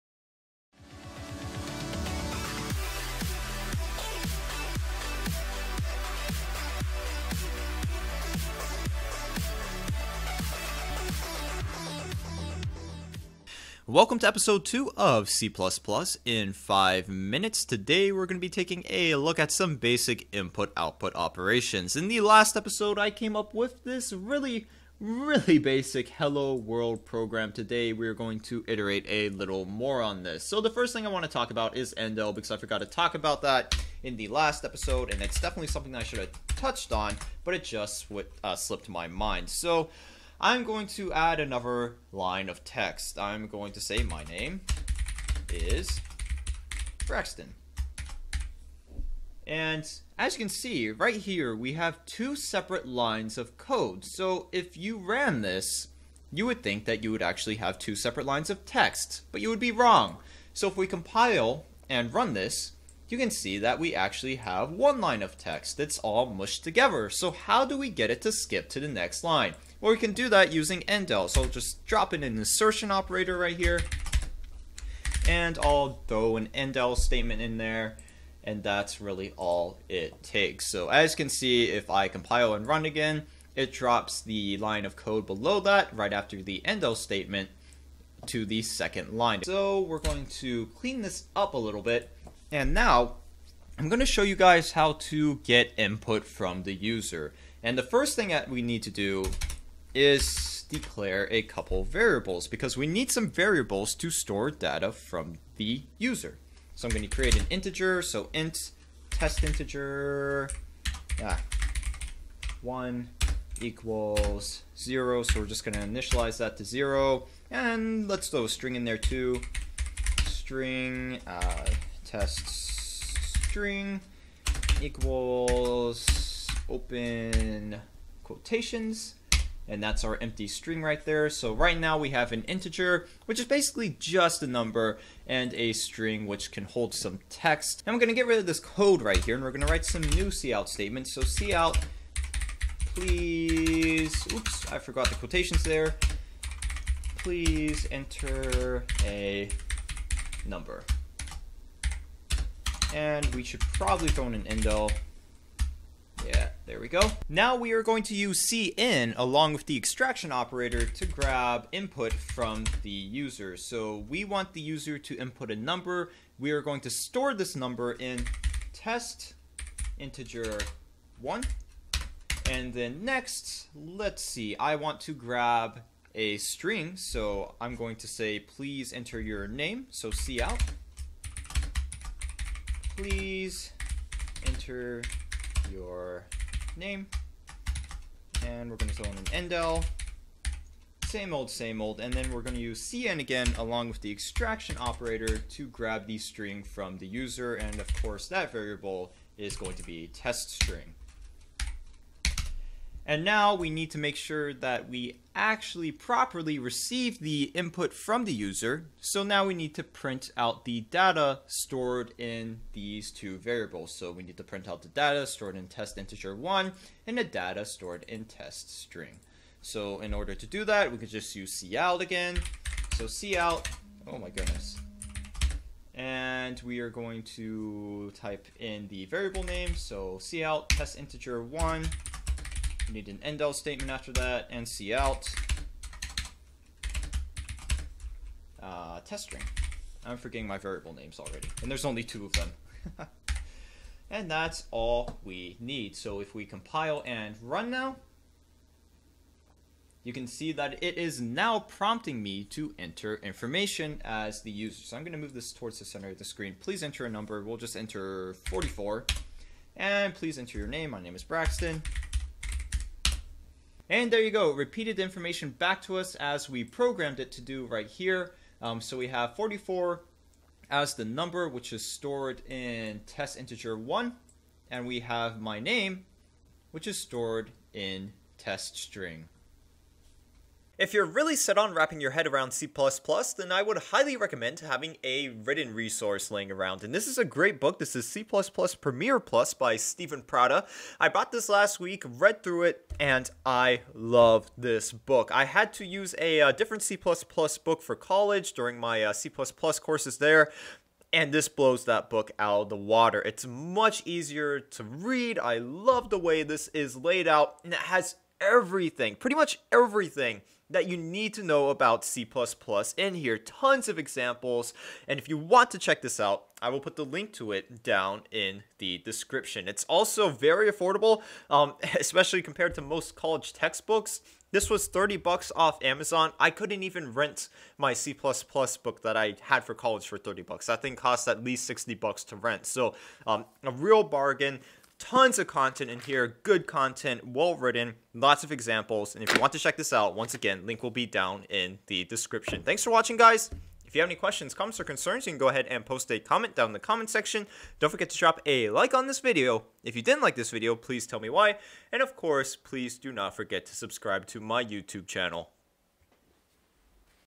C++ in 5 EP2: Basic Input and Output Using cin and cout
Today I am going to demonstrate how to receive input from the user using "cin" and then repeat the input back using "cout" in C++.  I will also talk a little about "endl" because I forgot to do that in the last video.

C++ Primer Plus

Support us!